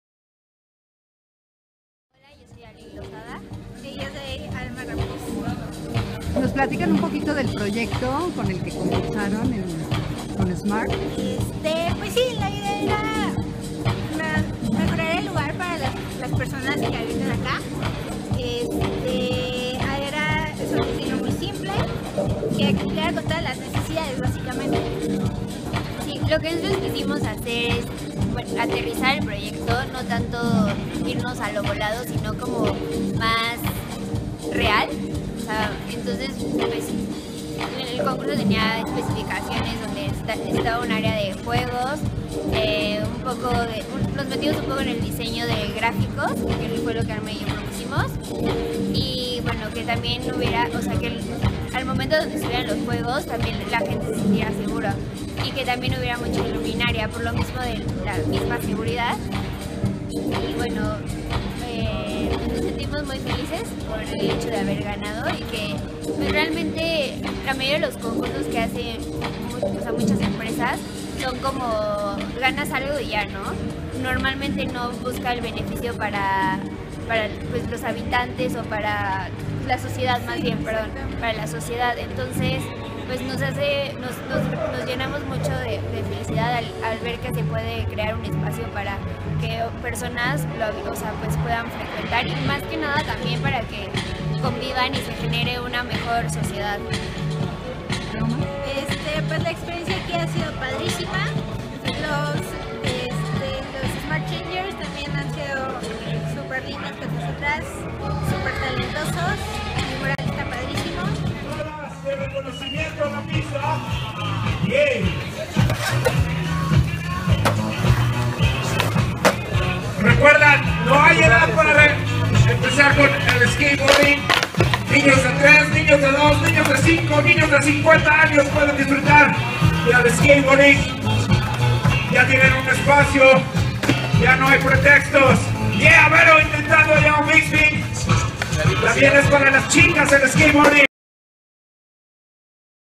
Concurso de Creatividad smart
Sin duda smart es el auto urbano por excelencia, pensado y concebido para vivir y disfrutar la ciudad como en ningún otro auto. Es por ello que la marca busca contribuir a que la ciudad sea un mejor lugar, mediante la filosofía de FORaNewUrbanJoy, de la cual se desprende el Concurso de Creatividad, que en su décimo primera edición tuvo como objetivo la recuperación de un espacio urbano.